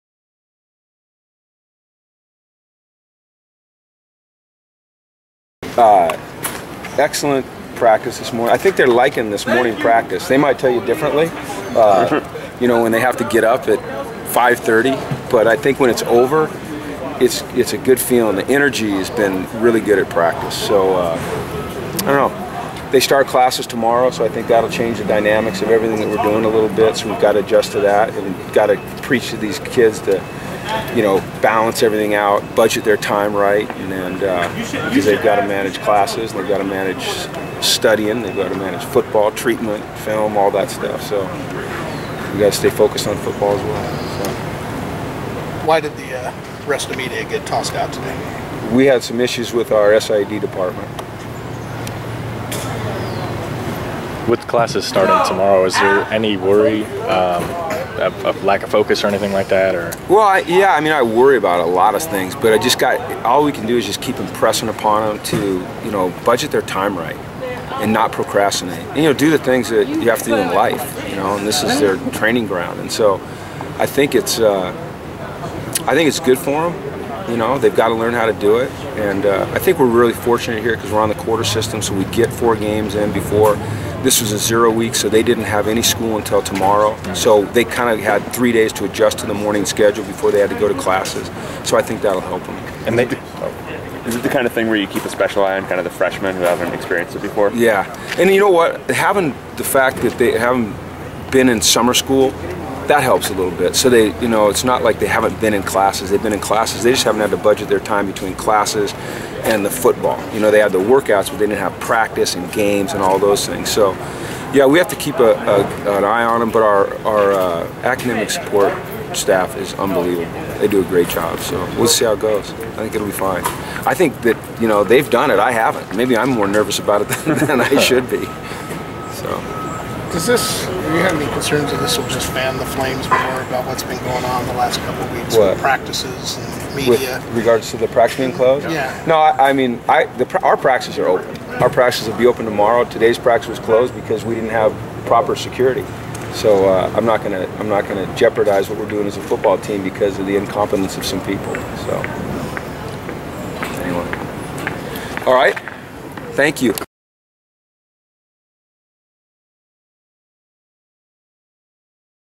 BSR TV: Jim Mora, Wednesday Practice
UCLA head coach Jim Mora spoke with reporters after Wednesday's closed practice.